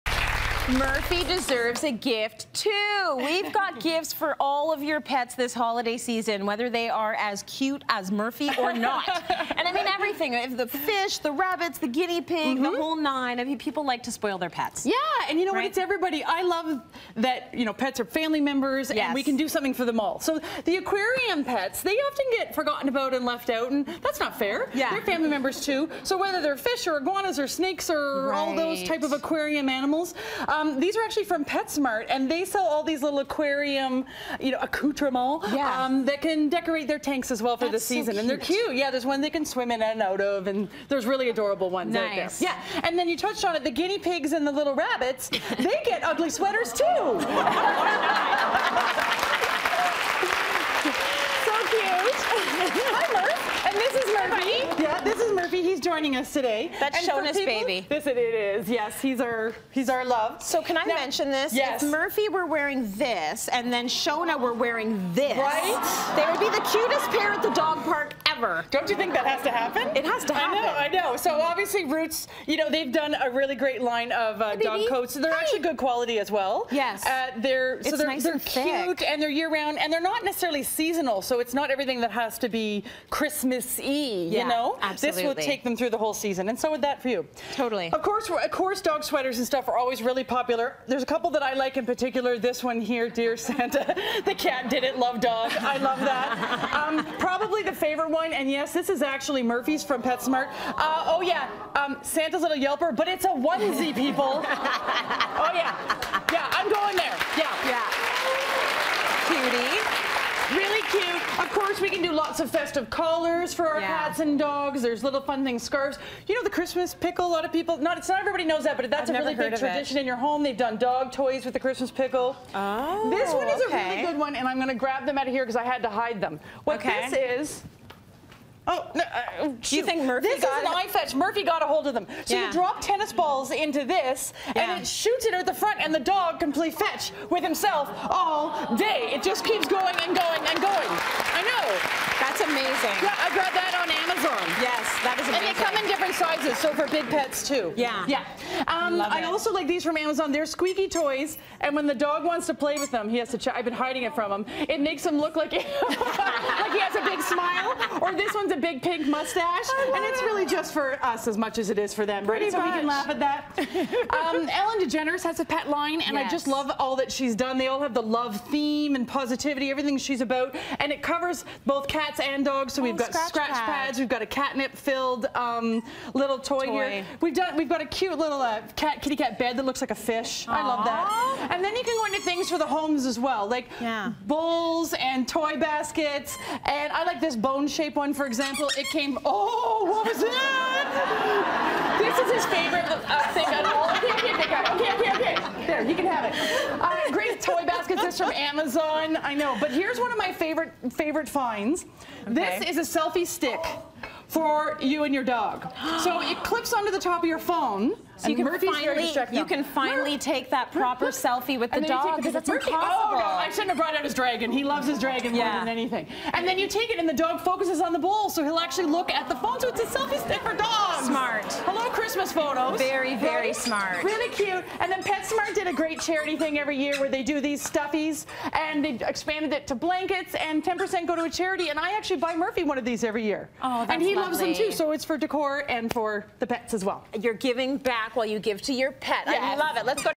11 adorable holiday gifts for pets and pet-lovers
Lifestyle expert Shoana Jensen has the perfect holiday gift for all of your pets this season, everyone from the goldfish to rover!